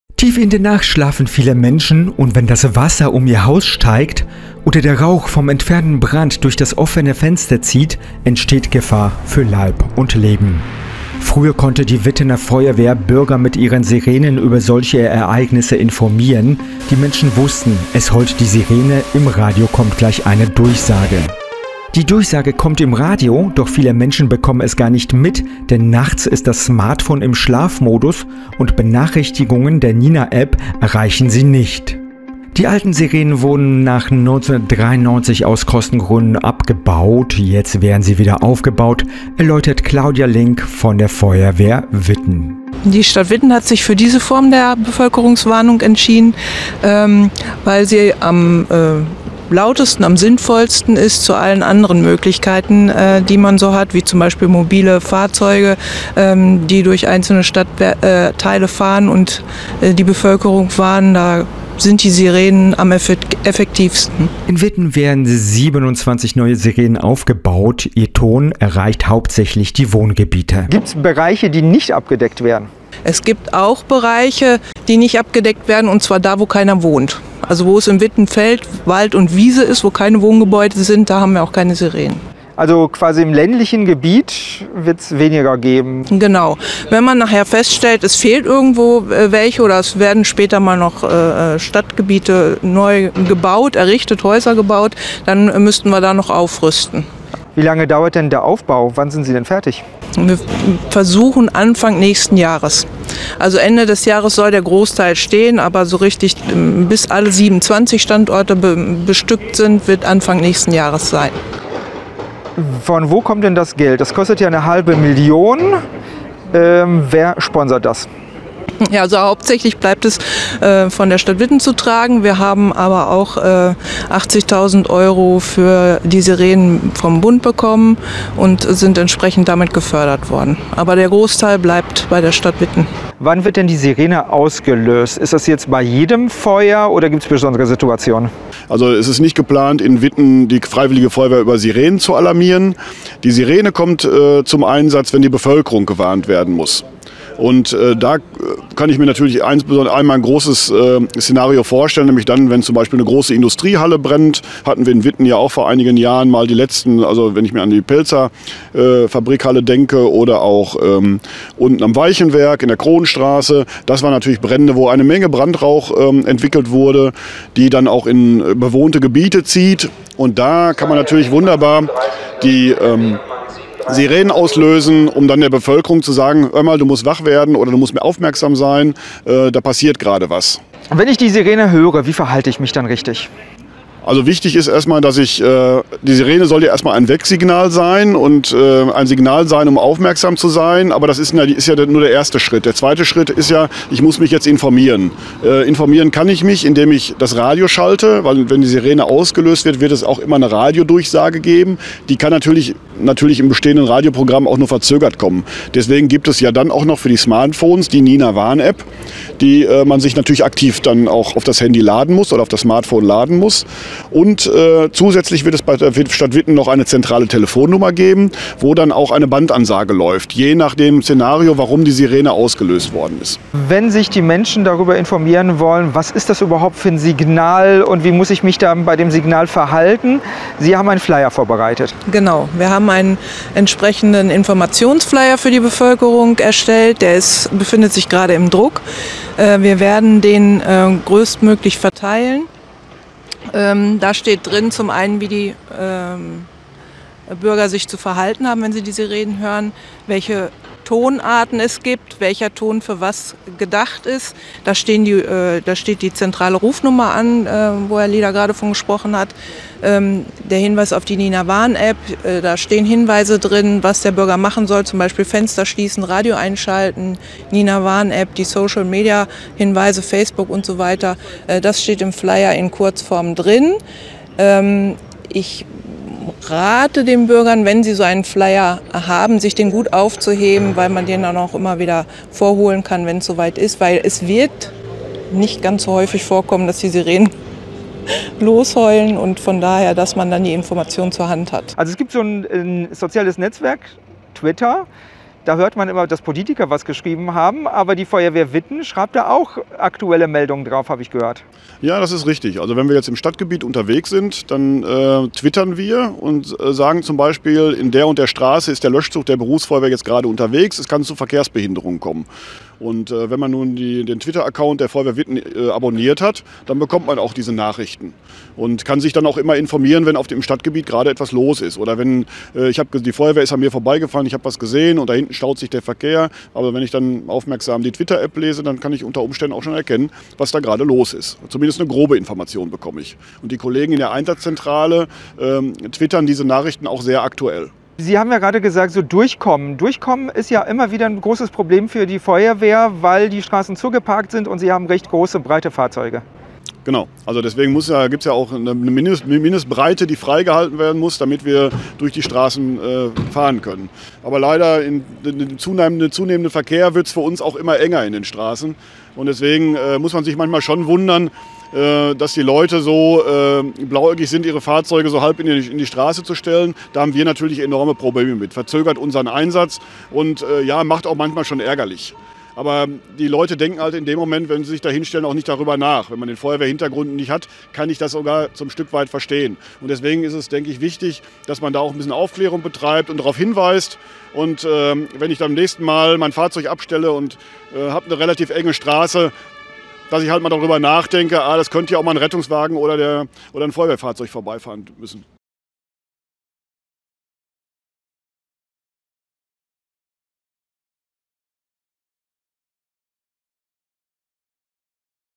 Neue Warnsirenen der Feuerwehr Witten
Tiefe in der Nacht schlafen vielen Menschen und wenn das Wasser um Ihr Haus steigt oder der Rauch vom entfernten Brand durch das offene Fenster zieht, entsteht Gefahr für Leib und Leben. Früher konnte die Wittener Feuerwehr Bürger mit ihren Warnsirenen über solche Ereignisse informieren. Die alten Sirenen wurden nach 1993 aus Kostengründen abgebaut. Die Wittener Feuerwehr hat angefangen neue Sirenen an 27 Standorten im Stadtgebiet aufzubauen. Ihr Ton erreicht hauptsächlich die Wohngebiete. Am Donnerstag (15.7.) präsentierte die Feuerwehr ihre erste vollfunktionierende Sirene auf dem Gebäude der Hauptfeuerwache an der Dortmunder Straße. Auf Grund des Hochwassers auf der Ruhr wurde die Sirene jedoch nicht vorgeführt, um die Menschen nicht unnötig zu verunsichern. Im Frühjahr 2022 soll der Ausbau abgeschlossen sein. Im Video sprechen wir mit Claudia Link, der Abteilungsleiterin Allgemeine Gefahrenabwehr und Dirk Lieder, dem stellv. Leiter der Wittener Berufsfeuerwehr.